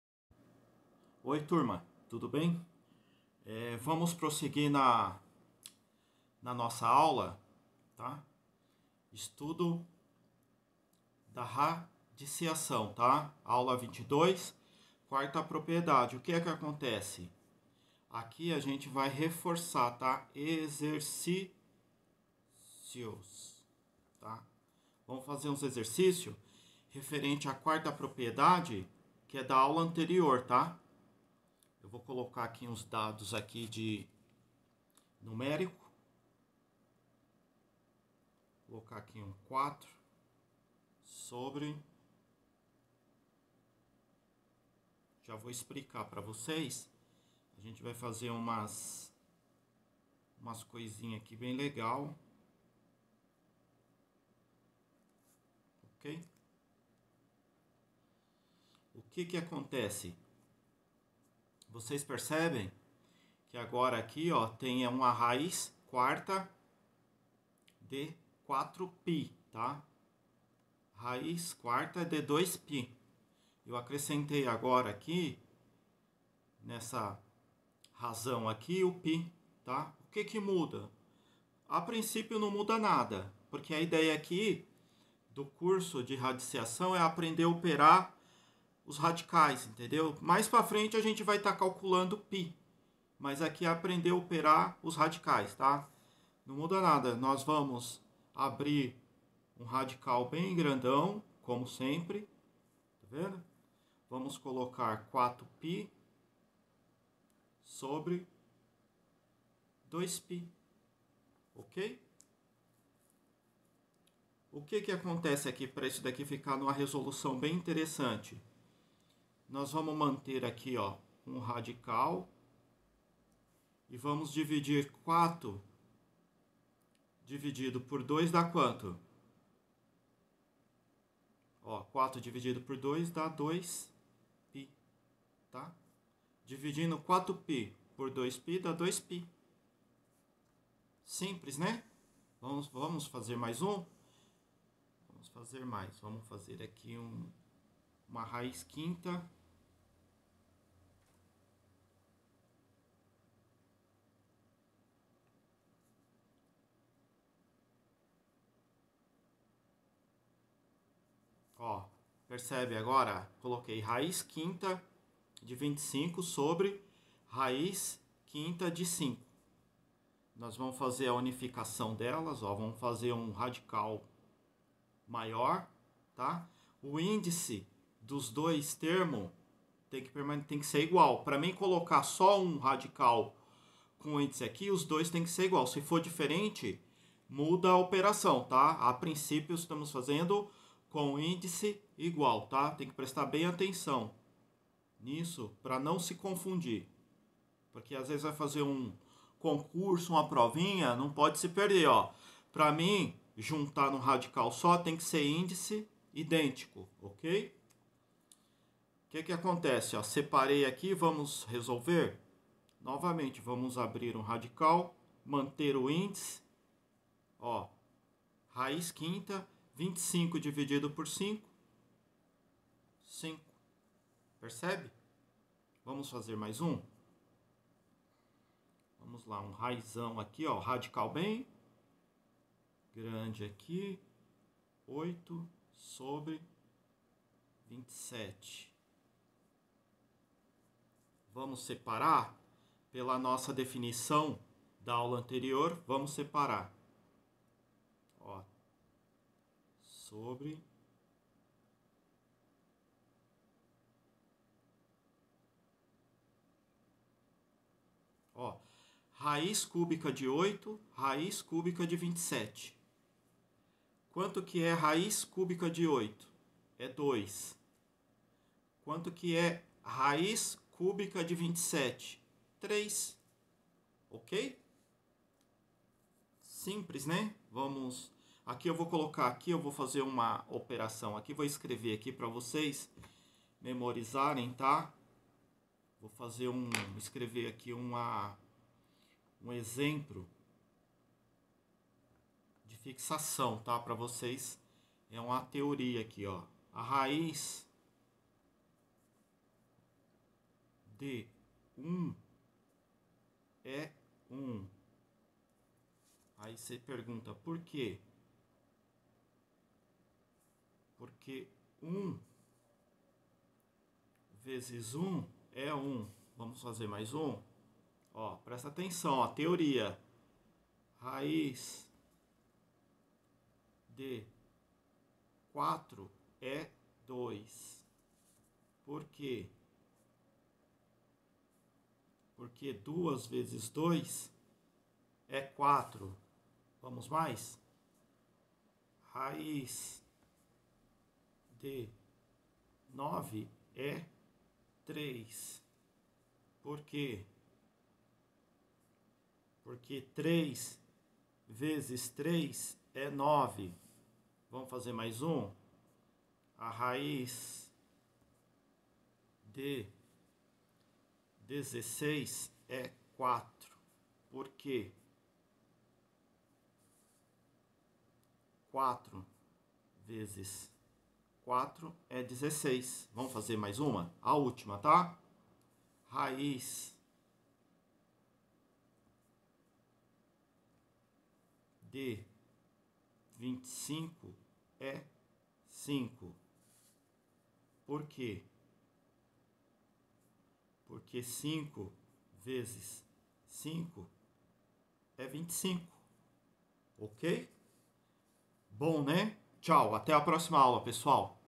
ESTUDO DA RADICIAÇÃO: AULA 22 - EXERCÍCIOS – 4ª PROPRIEDADE.
EXERCÍCIOS.
Propriedade 4: Quociente de raízes de índices iguais

De maneira análoga à 4ª propriedade, afirma que a divisão entre duas raízes de índices iguais é igual à raiz de mesmo índice da divisão dos quocientes.